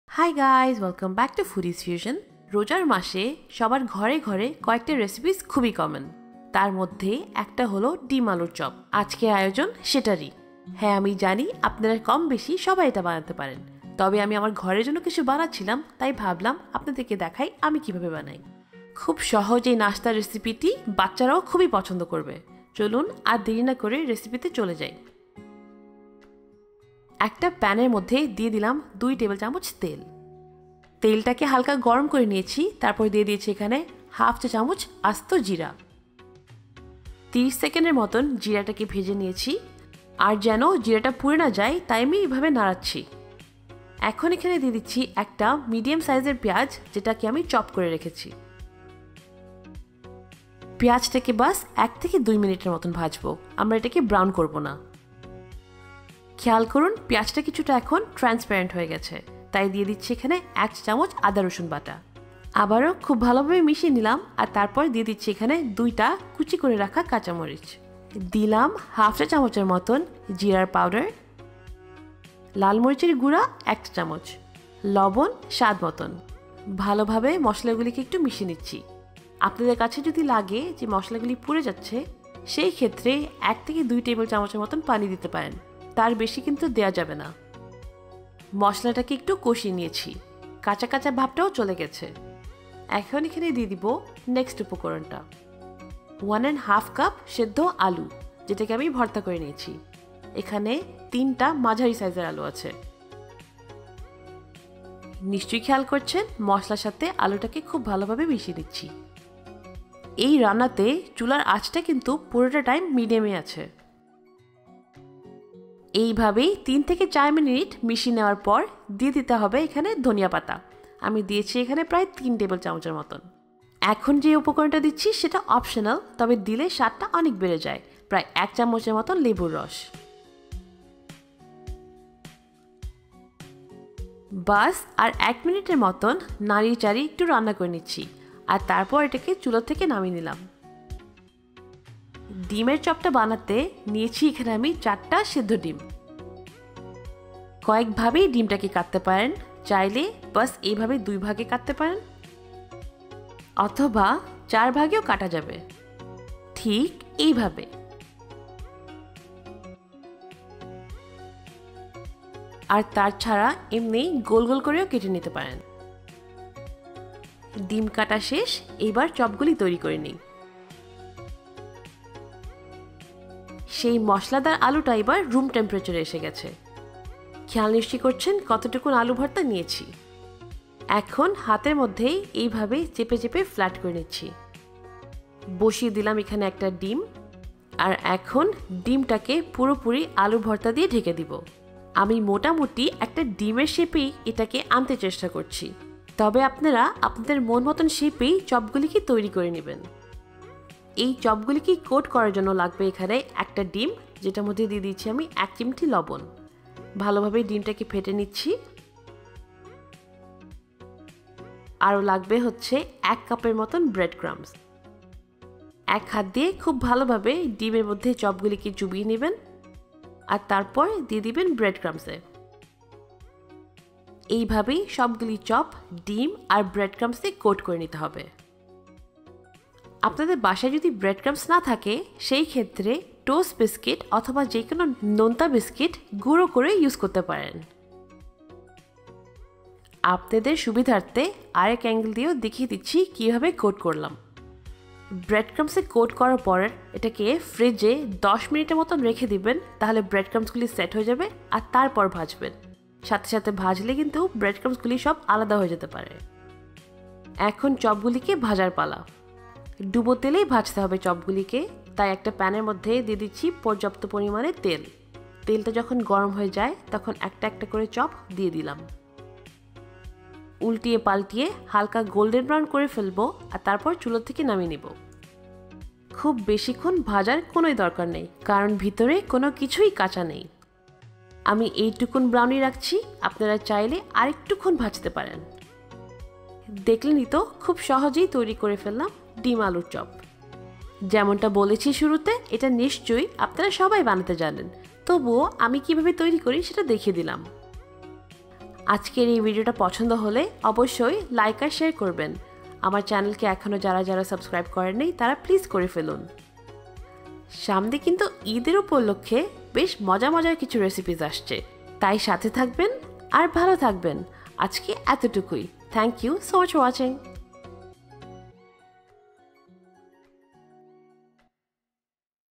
ডিম আলুর চপ তৈরির খুবই সহজ ইফতার রেসিপি | Egg Chop - Dim Aloo Chop | Easy Egg Potato Kabab for Iftar
How to make Egg Chop - Dim Alur Chop - Easy Egg Potato Kabab Recipe in bengali